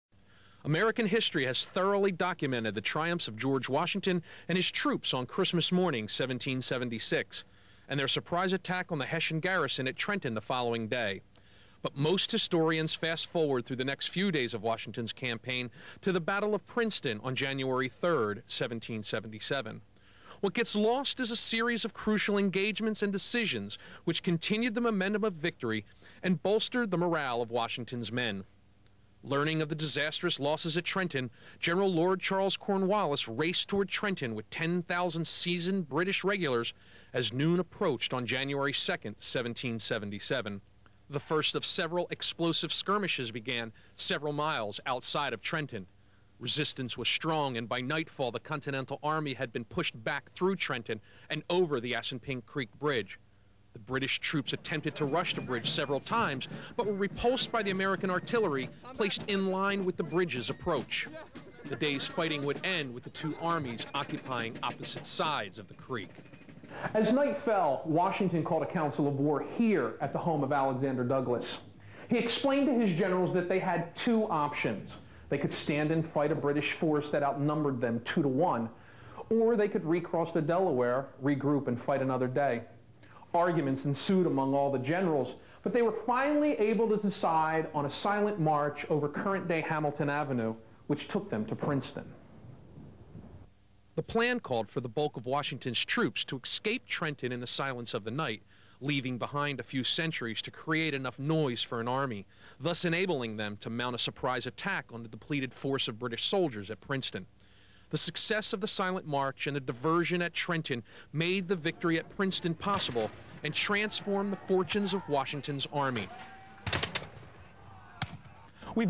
Second Battle
Often overlooked this second encounter between American and British forces in Trenton provided George Washington with another important victory.